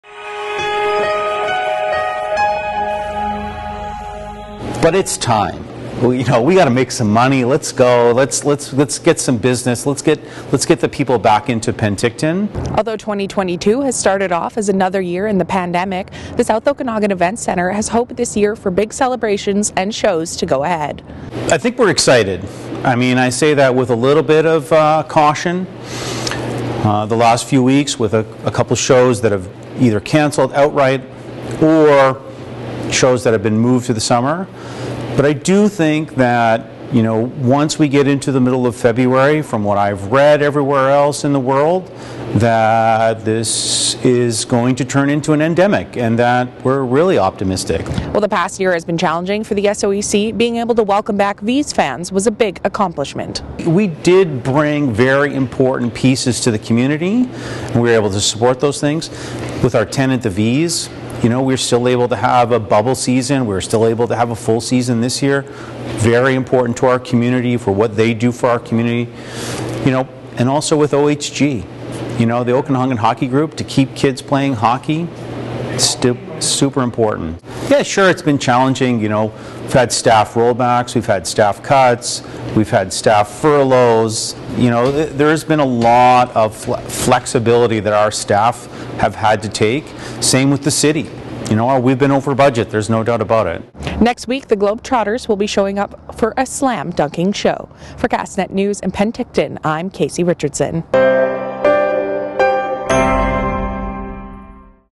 Hope for big events and concerts
Although 2022 has started off as another year in the pandemic, the South Okanagan Events Centre has hope for big celebrations and shows to go ahead.